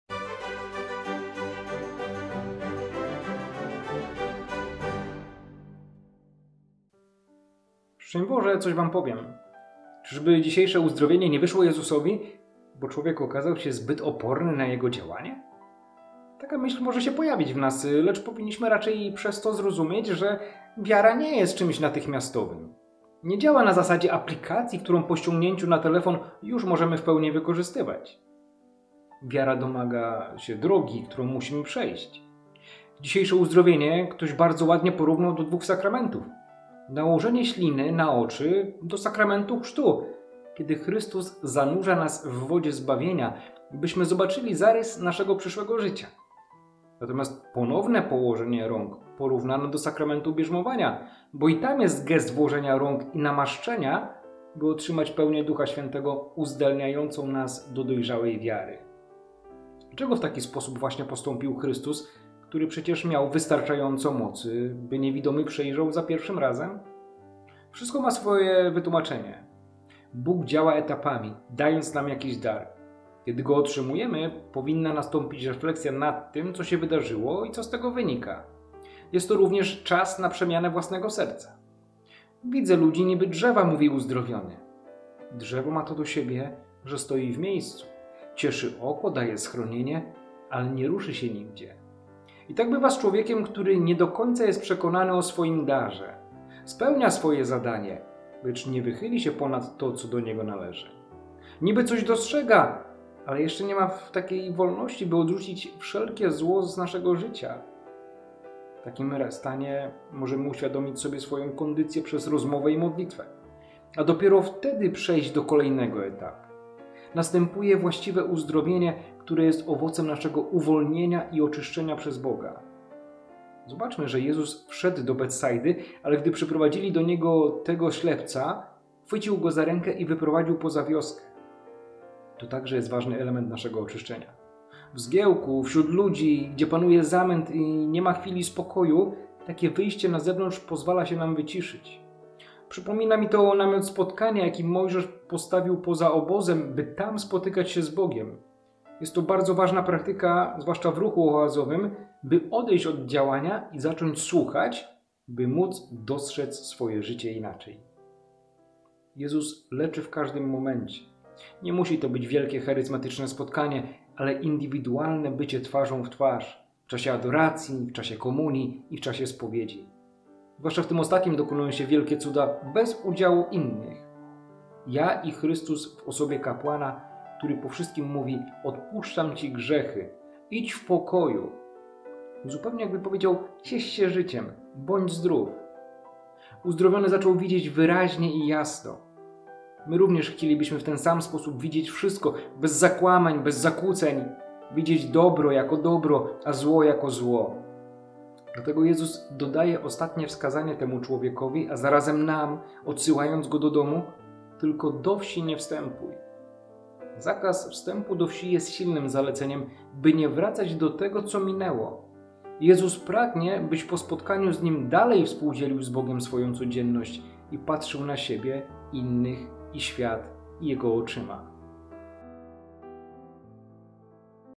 coswampowiem Mk 8, 22-26 * 15 lutego 2023
coswampowiem Mk 8, 22-26 * 15 lutego 2023 Komentarz do Ewangelii
Bóg działa etapami, dając nam jakiś dar. Kiedy go otrzymujemy, powinna nastąpić refleksja nad tym, co się wydarzyło i co z tego wynika. Jest to również czas na przemianę własnego serca.